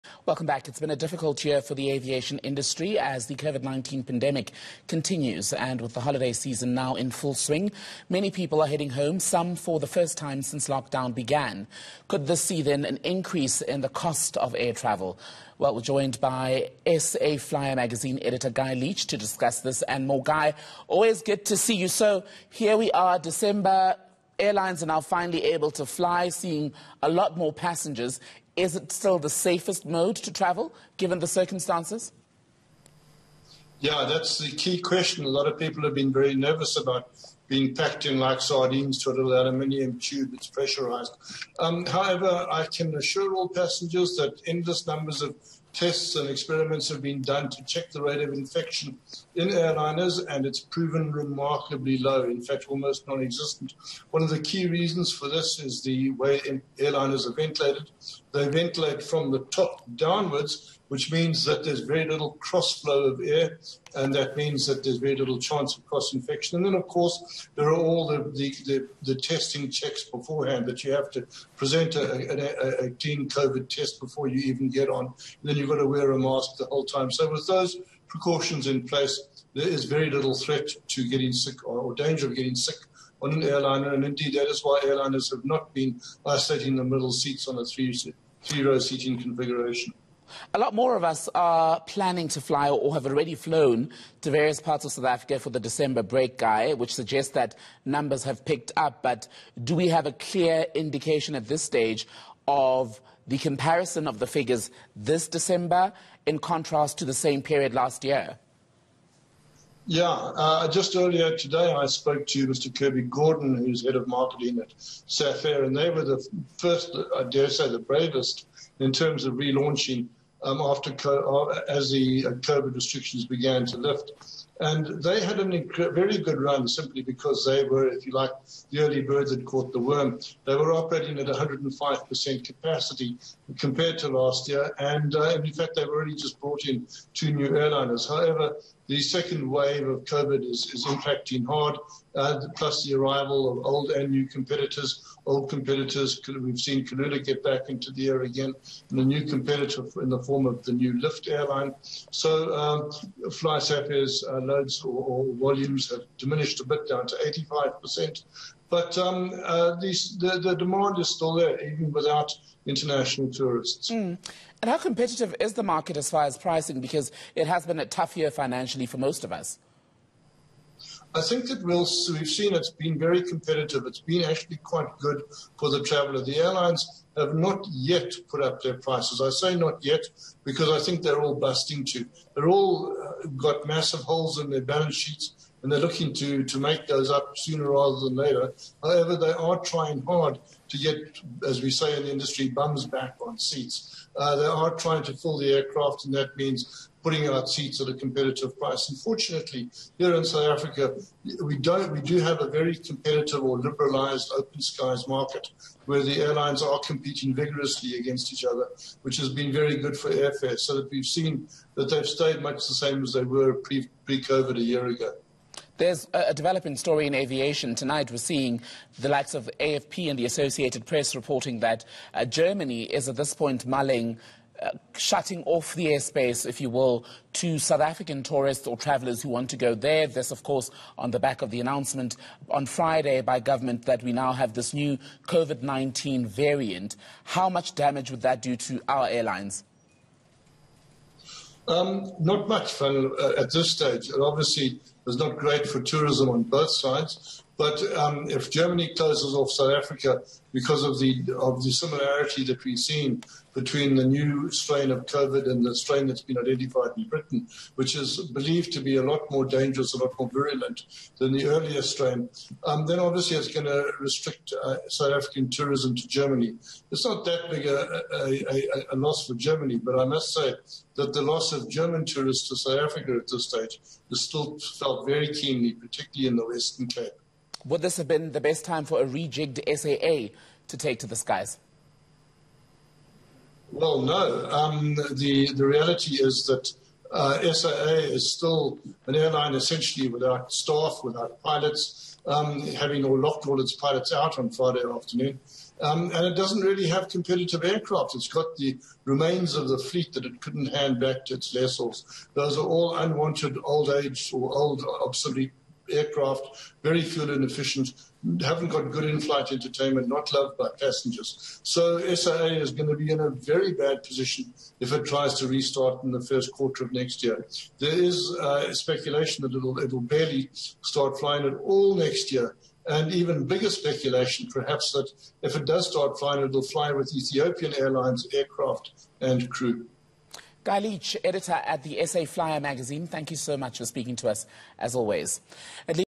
Aviation industry recovery
SA Flyer Magazine Editor, Guy Leitch spoke with eNCA's Thembekile Mrototo. Courtesy of DStv403